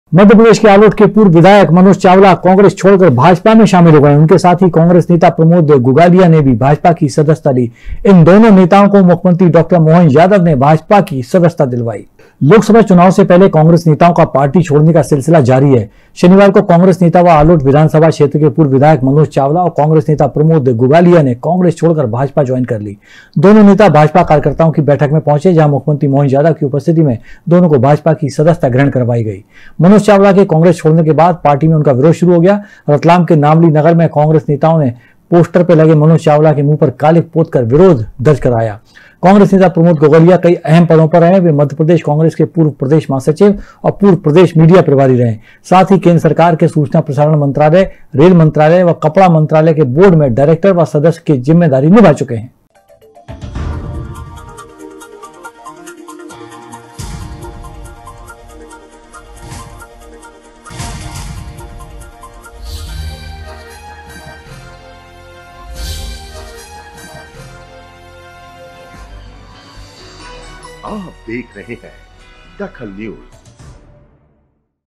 DAKHAL NEWS पूर्व विधायक मनोज चावला भाजपा में
पूर्व विधायक मनोज चावला भाजपा में
प्रमोद गुगालिया भी भाजपाई हुए